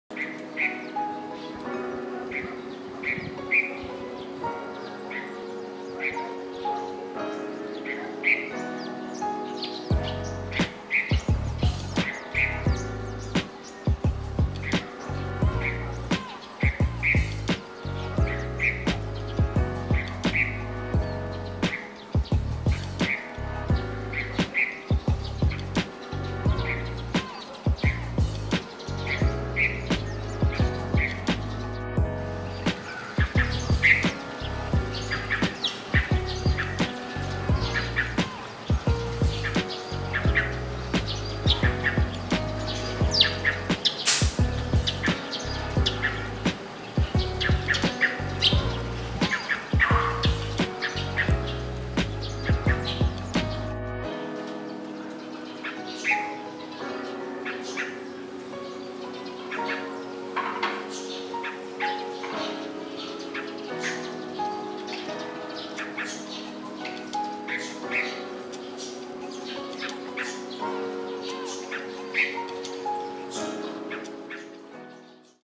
Yellow-vented Bulbul
A singing Yellow-vented Bulbul perched on a tree branch at home.

Since we've been on lockdown for quite some time, I tried bird watching in the morning when it's not so hot and the birds are active. Luckily I was able to get close enough to take photos and videos of this Yellow-vented Bulbul.